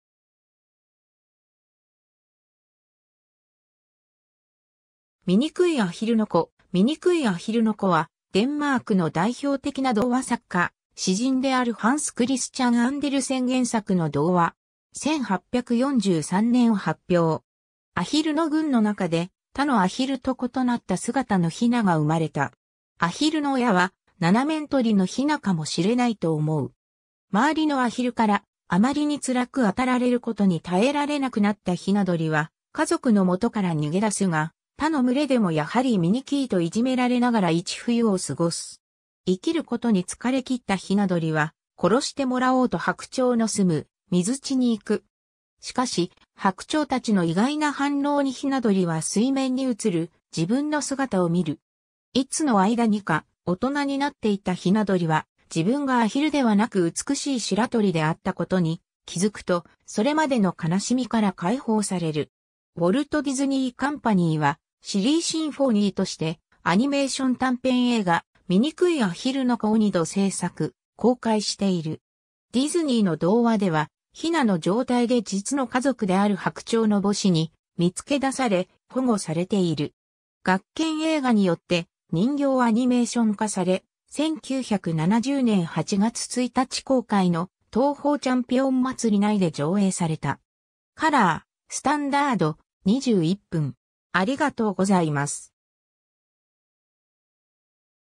みにくいアヒルの子
みにくいアヒルの子 「みにくいアヒルの子」（みにくいあひるのこ、丁: Den grimme Ælling）はデンマークの代表的な童話作家・詩人であるハンス・クリスチャン・アンデルセン原作の童話。
1843年発表。
 アヒルの群の中で、他のアヒルと異なった姿のひなが生まれた。
アヒルの親は、七面鳥のひなかもしれないと思う。
周りのアヒルから、あまりに辛く当たられることに耐えられなくなったひな鳥は家族の元から逃げ出すが、他の群れでもやはり醜いといじめられながら一冬を過ごす。
生きることに疲れ切ったひな鳥は、殺してもらおうと白鳥の住む水地に行く。
しかし、白鳥たちの意外な反応にひな鳥は水面に映る自分の姿を見る。
いつの間にか大人になっていたひな鳥は、自分がアヒルではなく美しい白鳥であったことに気付くと、それまでの悲しみから解放される。
 ウォルト・ディズニー・カンパニーは、シリー・シンフォニーとしてアニメーション短編映画『みにくいあひるの子 (The Ugly Duckling)』を2度製作、公開している。
 ディズニーの童話では、ひなの状態で実の家族である白鳥の母子に見つけだされ、保護されている。
 学研映画によって人形アニメーション化され、1970年8月1日公開の「東宝チャンピオンまつり」内で上映された。
カラー、スタンダード、21分。